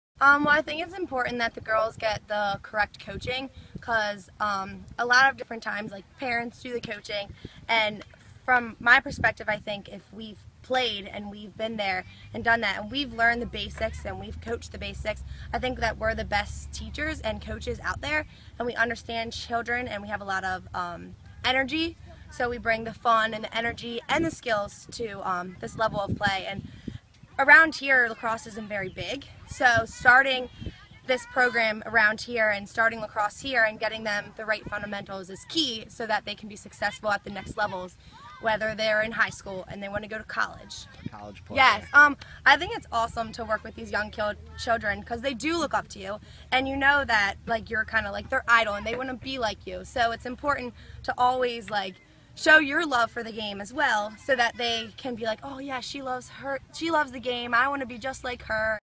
Former Towson Univ lax player @kleech2 talking about working the @marylandwlax at South Western HS
By: Mike Rubin
Published on: June 24, 2014